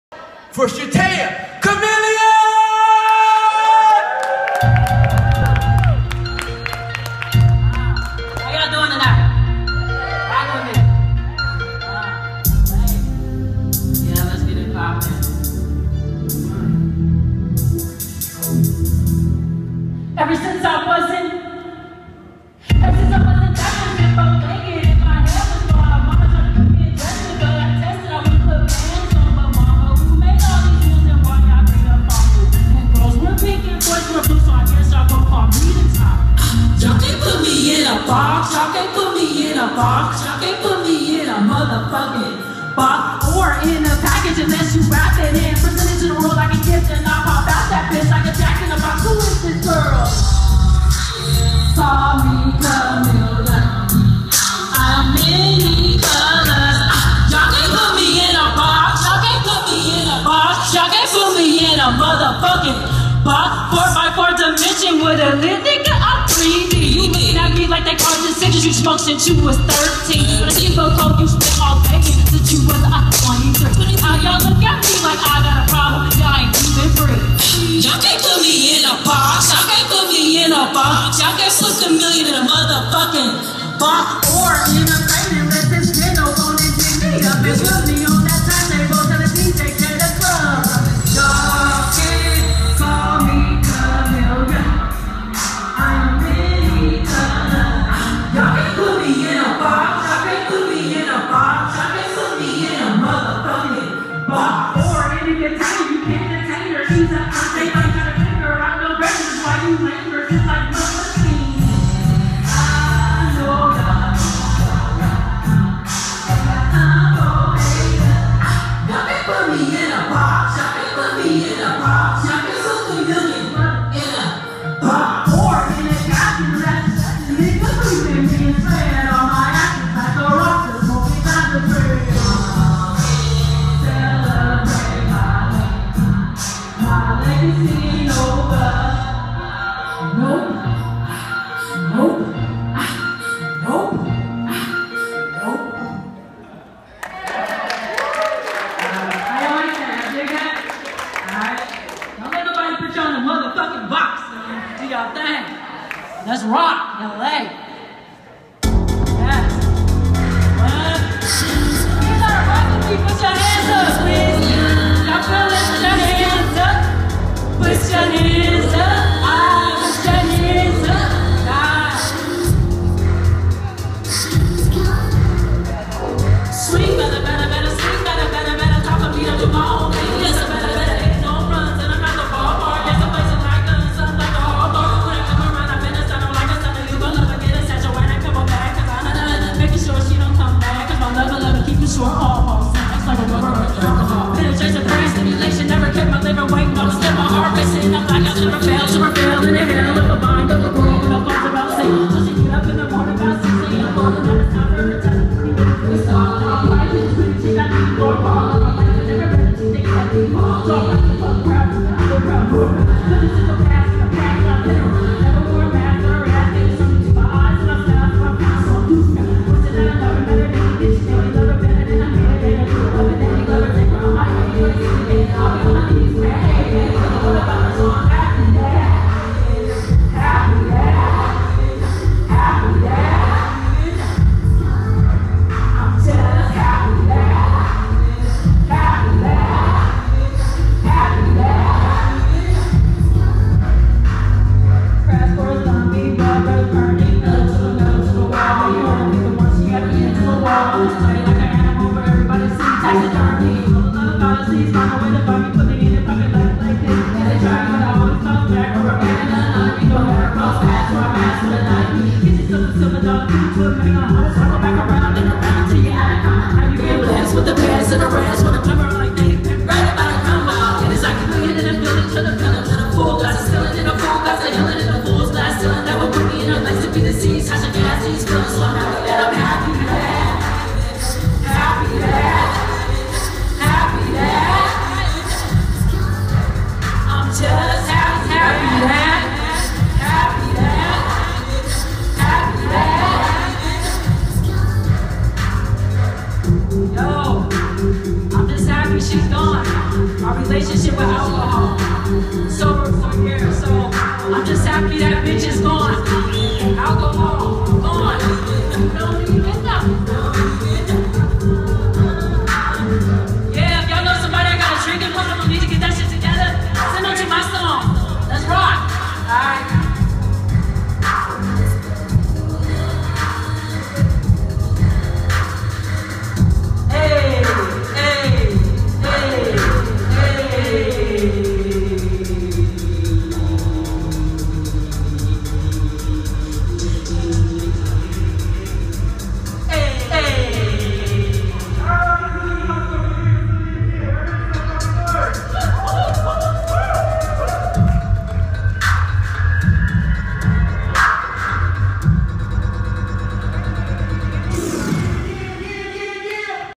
Shataya Kamilion 's first show!
Shataya Kamilion performing LIVE for the first time!